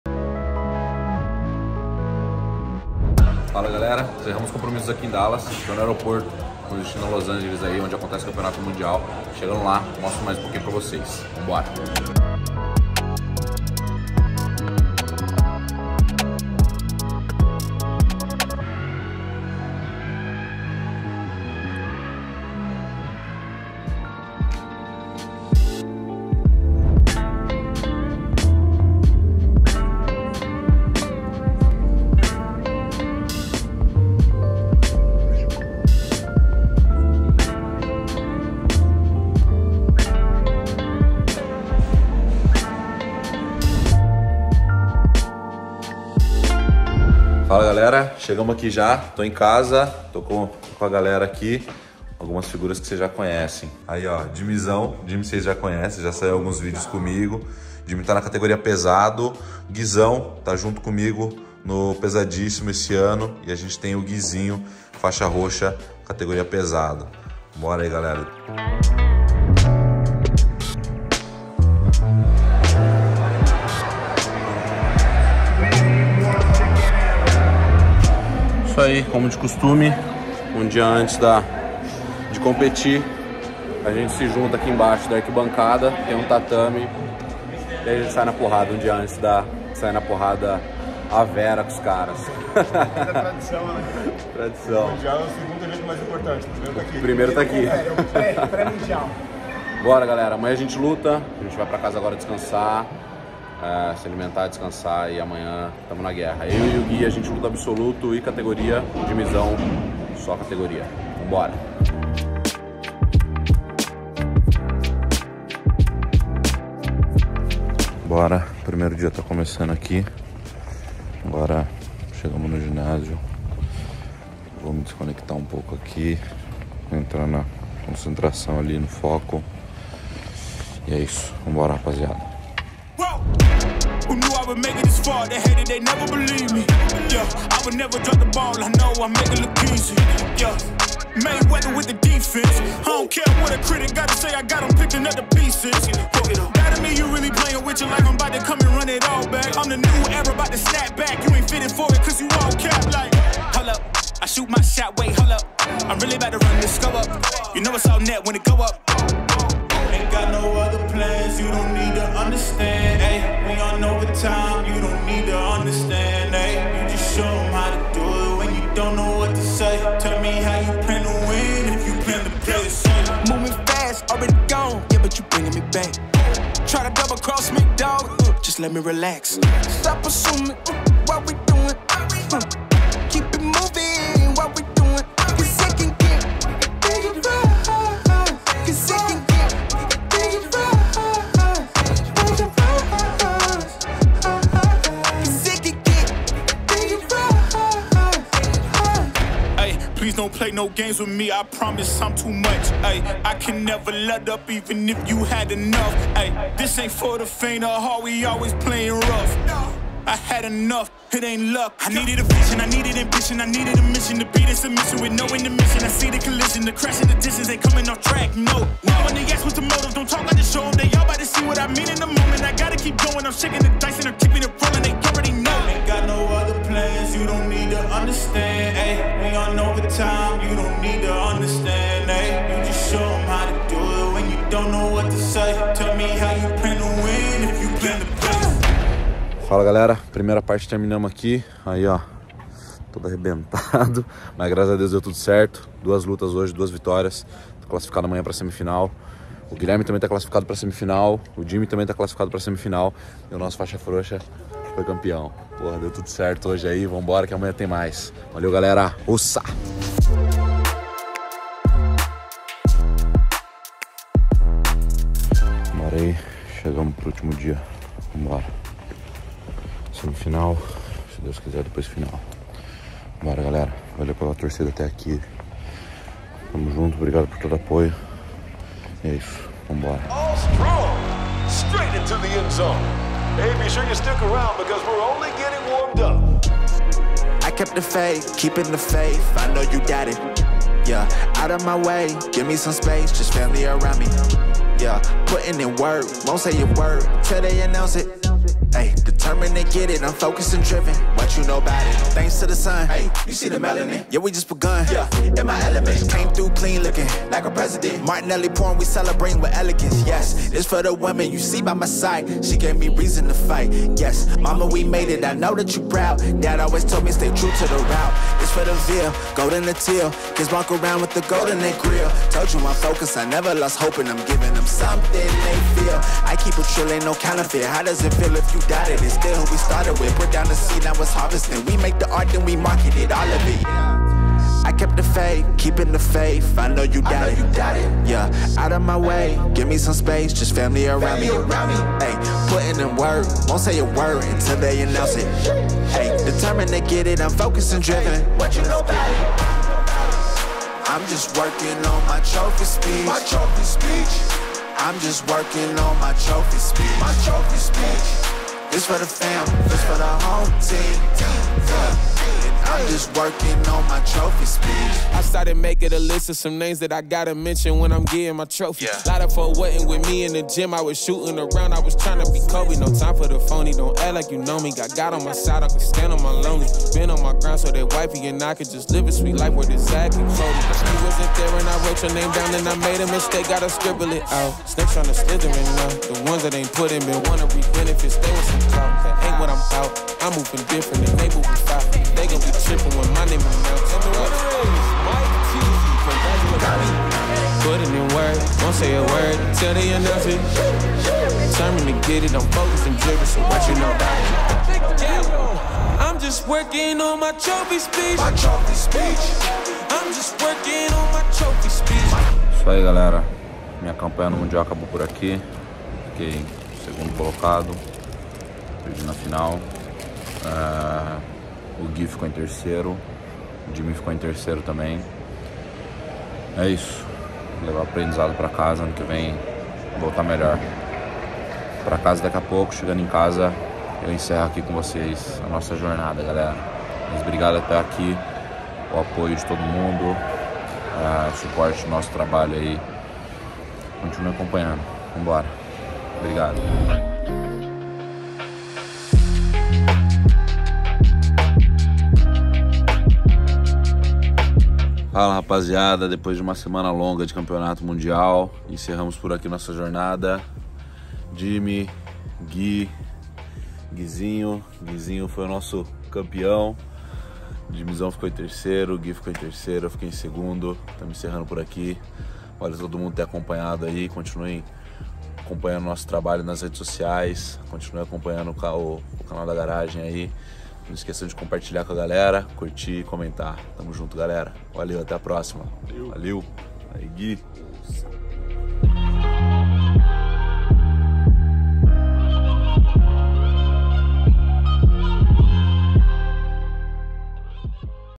MUNDIAL DE JIU-JITSU - Time Alliance (VLOG)
O time Alliance de Jiu-Jitsu do Brasil vai ao Mundial de Jiu-Jitsu em Los Angeles nos Estados Unidos.

Acompanhe no Vlog como foi!🔥🐗🔥